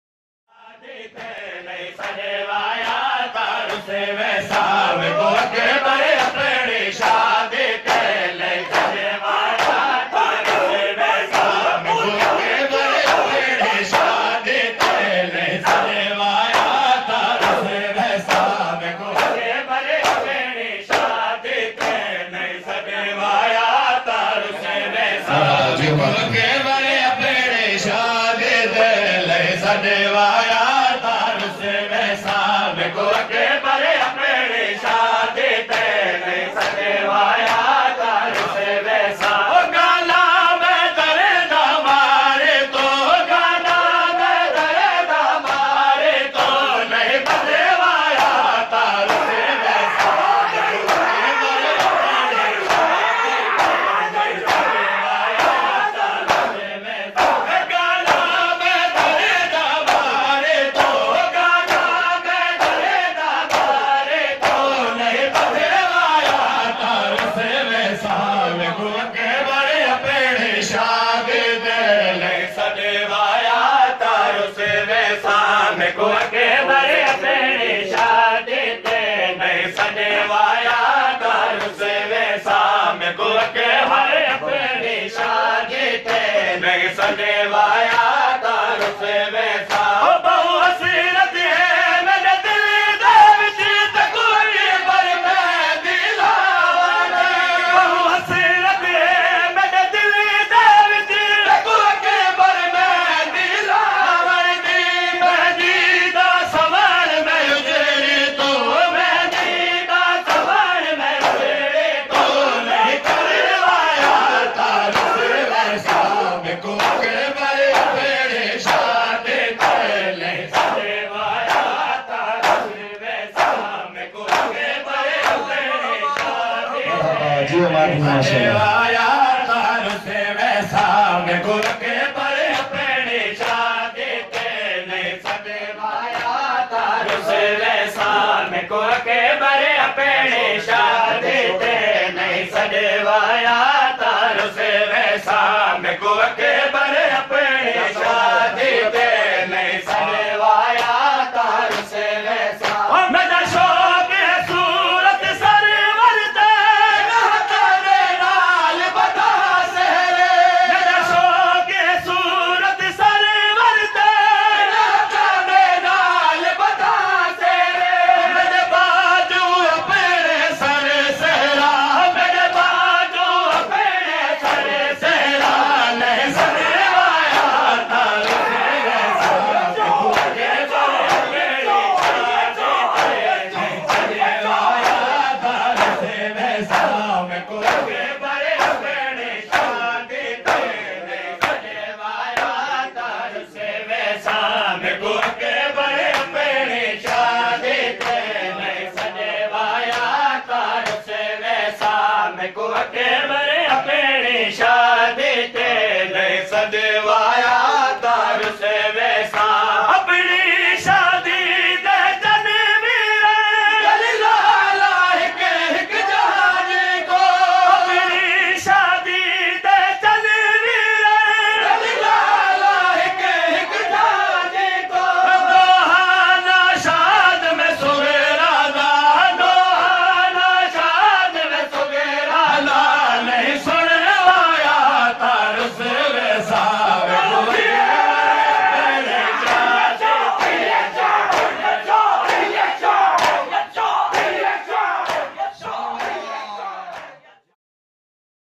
Shahid Hussain Qureshi.(Mekoon Akbar apni shadi te).
On. 31 December 2021.
26 Jamadiul awal1443.
At. Siyal Mohalla.
Mirpur Mathelo. Dist.Ghotki.